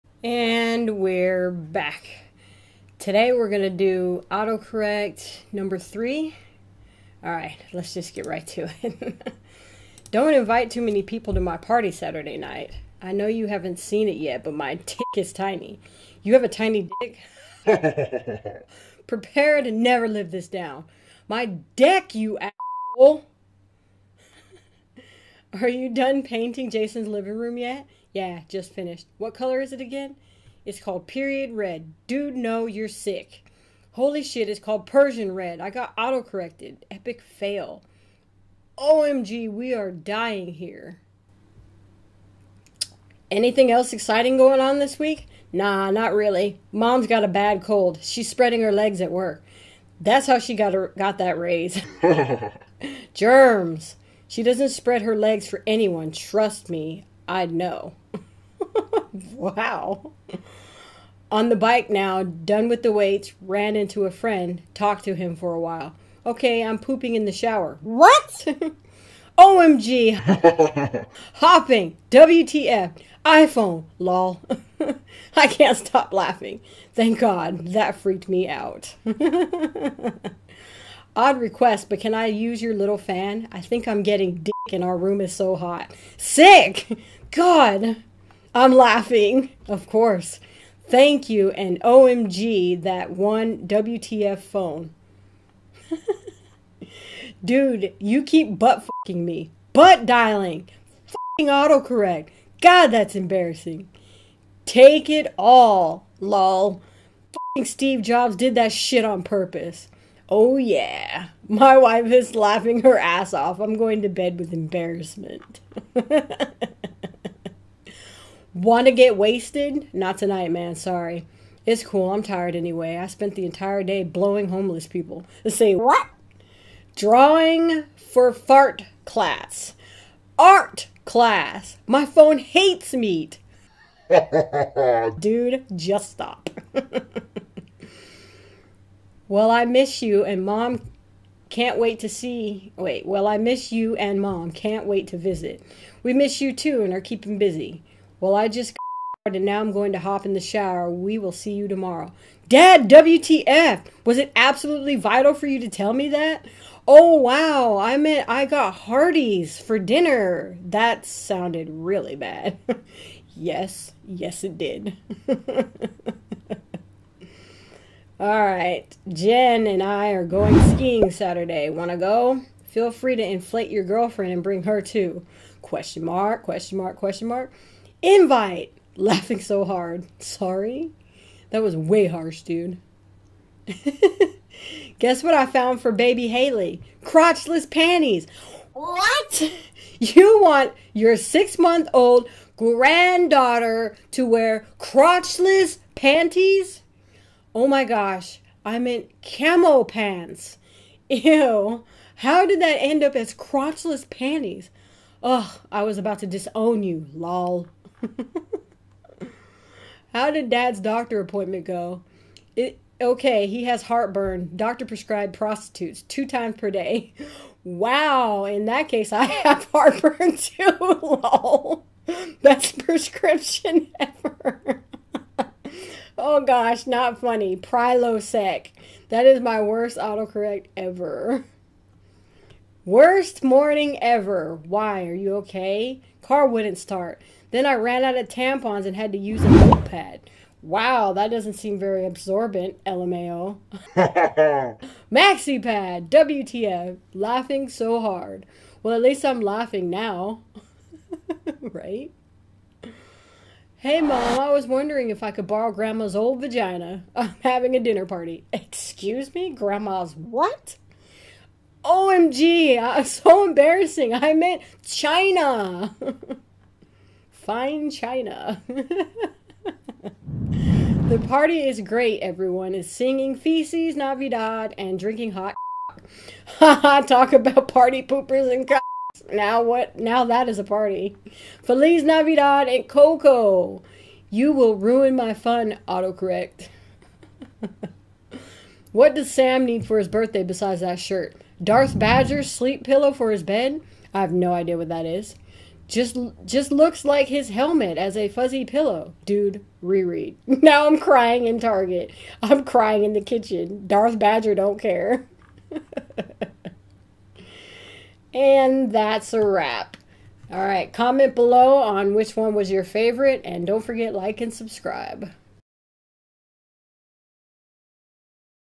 Funniest Autocorrects #3
In this video I react to some of the funniest autocorrects!